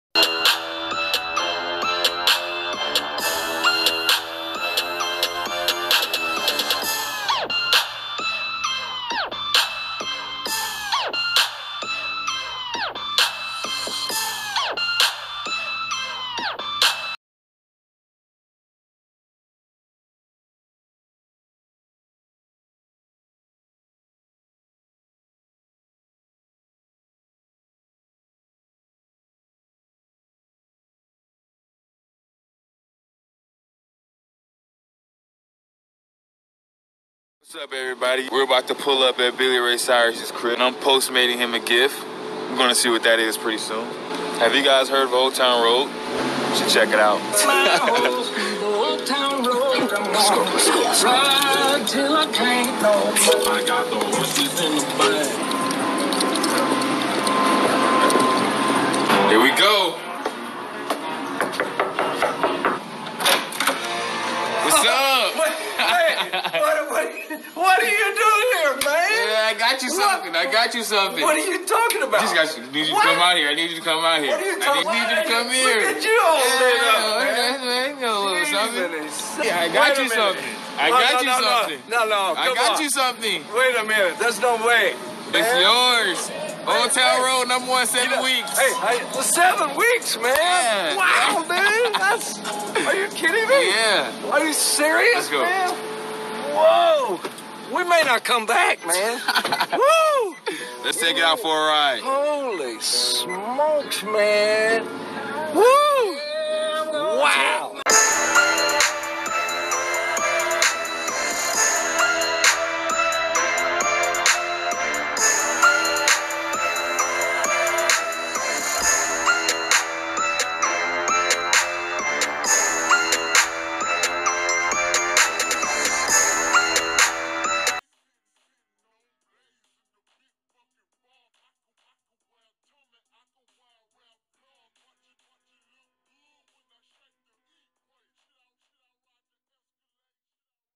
🔵🔴LIL NAS X BUYS BILLY RAY CYRUS A MASERATI🚗
Lil Nas X gifted Billy Ray Cyrus a gift as a token of appreciation for his support on X's track .